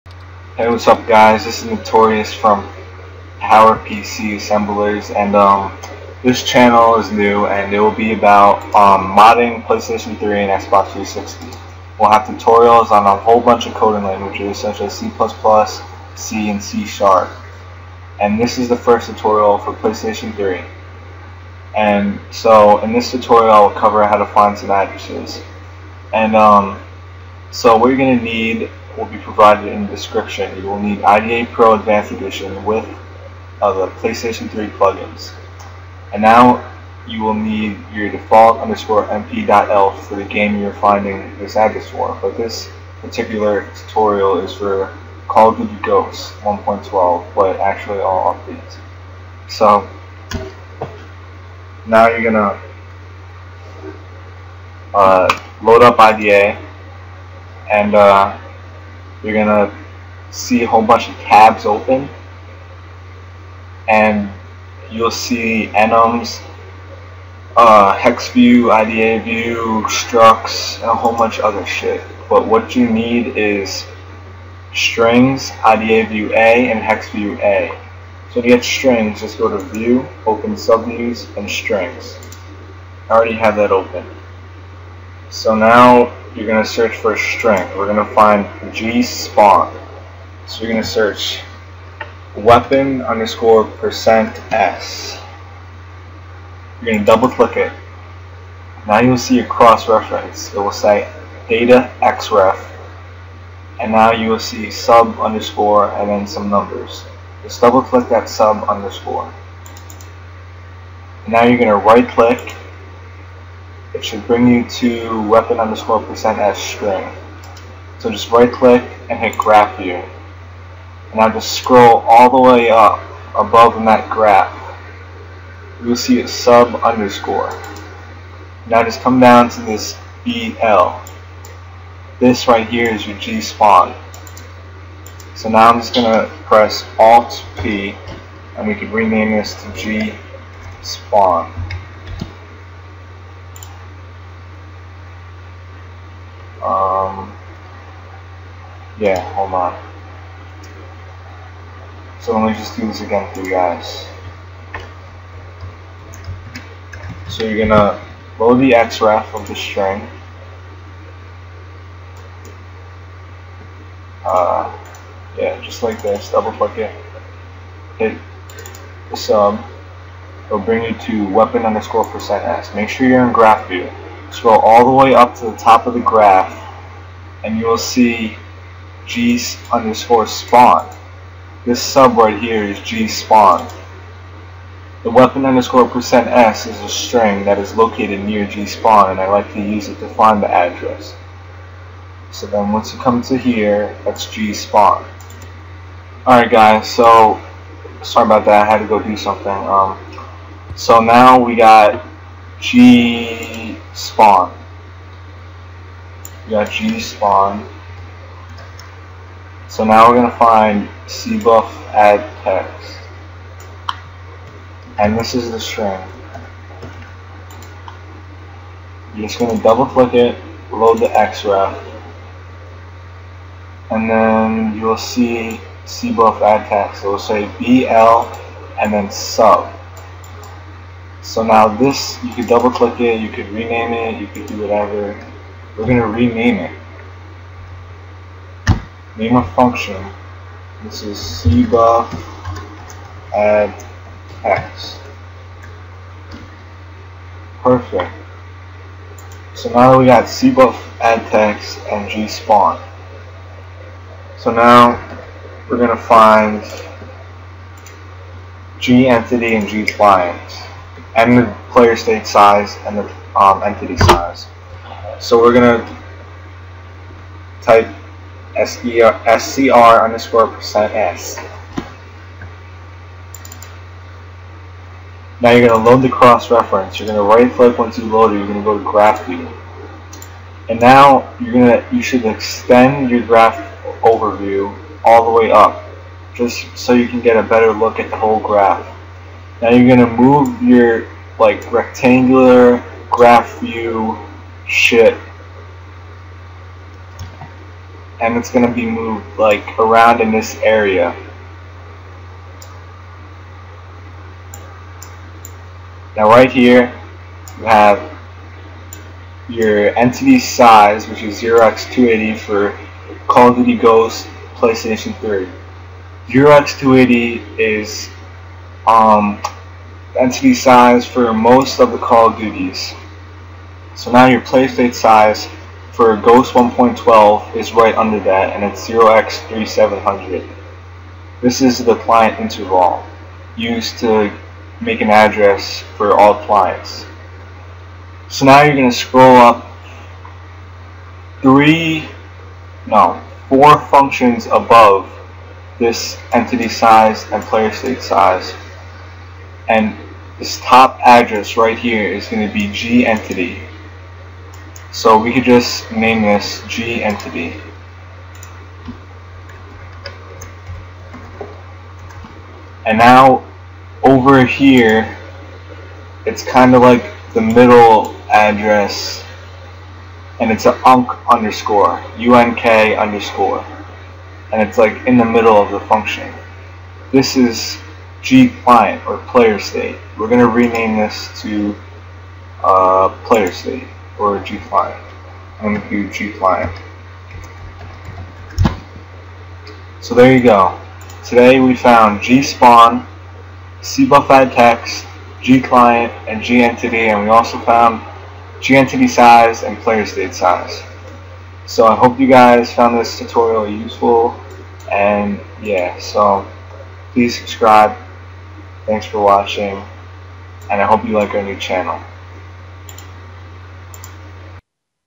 [PS3] How To Find Basic Addresses [IDA PRO]
Hey everyone this is our first tutorial !
This tutorial covers the following things:
How to find basic addresses in IDA PRO for PS3 such as G_Spawn, G_Client, G_Entity, Cbuf_Addtext

I hope you found this helpful